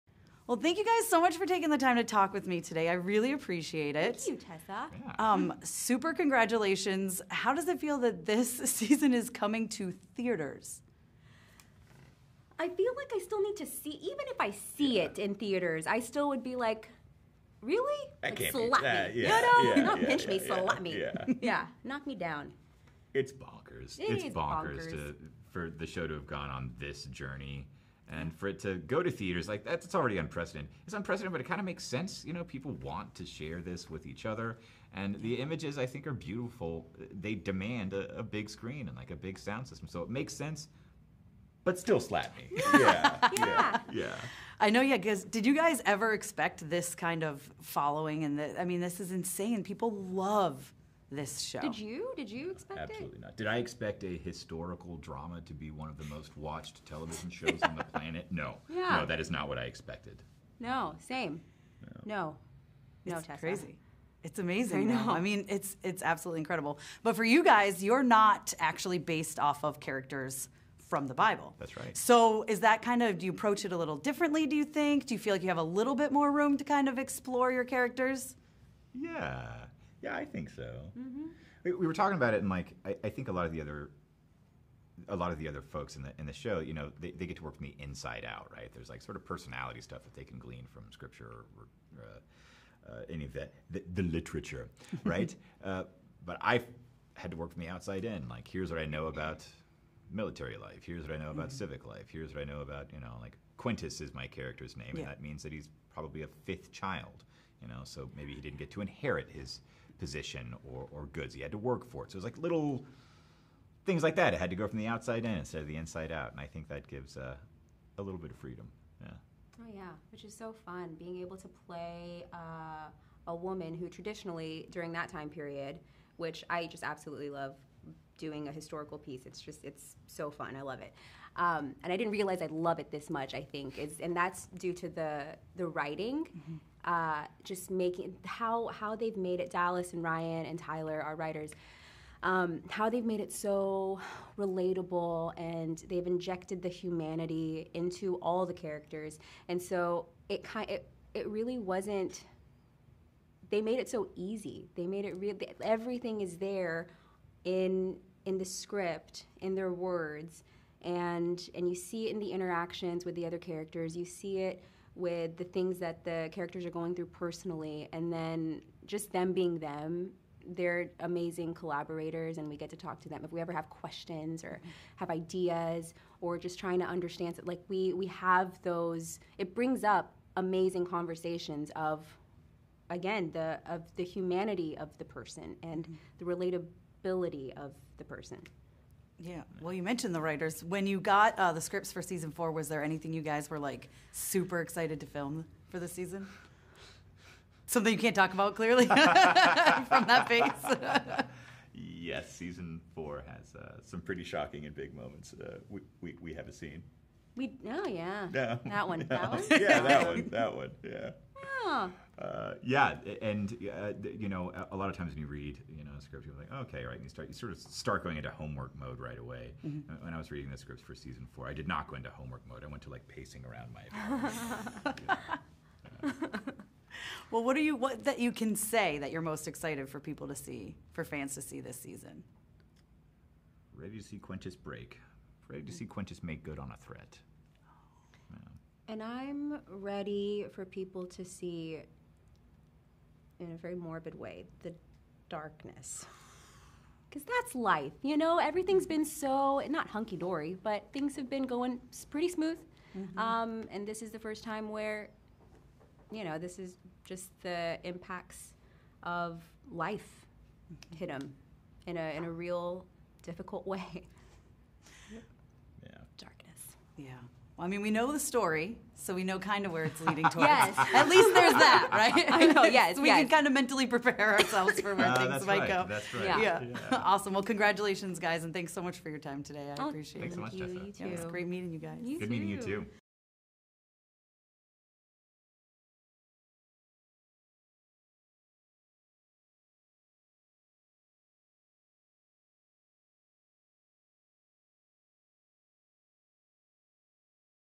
The Chosen Season 4 Interview: Yasmine Al-Bustami & Brandon Potter
Yasmine Al-Bustami & Brandon Potter talk all things The Chosen season 4 in this interview, including how they feel about it releasing in theaters, what it is like playing characters that are not in the Bible, and more.